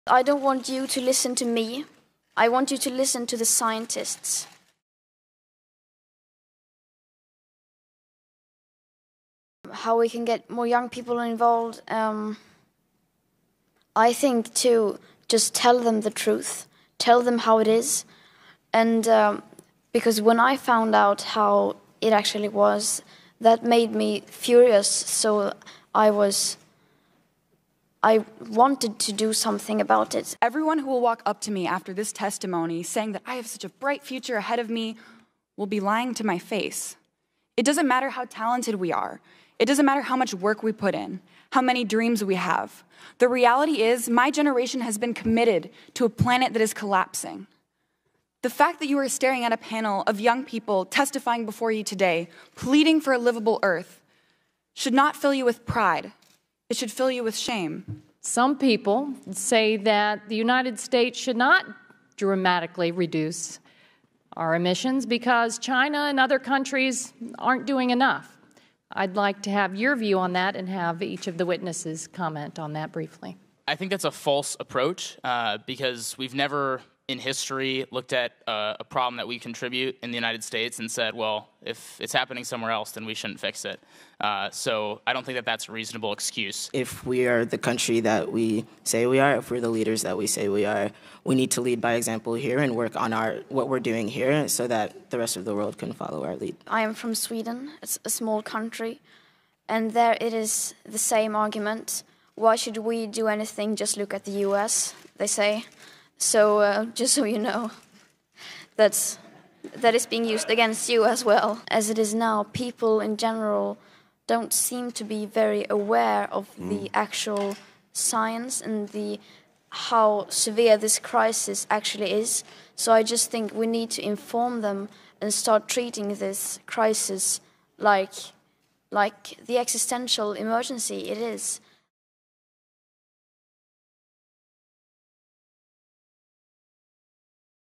「私ではなく科学を聞いて」10代の環境活動家らがアメリカ議会で証言
「私が言うことを聞くのではなく、科学者たちが言っていることを聞いてほしいのです」

16歳の環境活動家グレタ・トゥーンベリさん。アメリカ議会で他の10・20代の活動家とともに、温室効果ガス排出などが引き起こしている気候変動の危機について証言をしました。